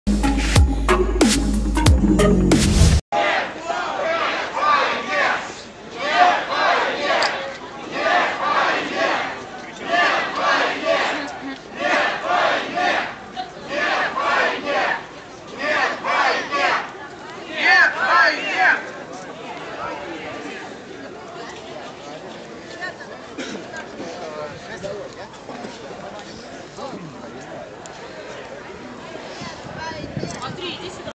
Марш Мира 15 марта 2014
в Москве больше 100.тыс россиян вышли на "Марш мира" чтобы поддержать Украину и выступить против ввода российских войск в Украину.

Максимальный РЕПОСТ!

- Online Свежие Новости в Мире ᵀᴴᴱ ᴼᴿᴵᴳᴵᴻᴬᴸ

Максимальный РЕПОСТ!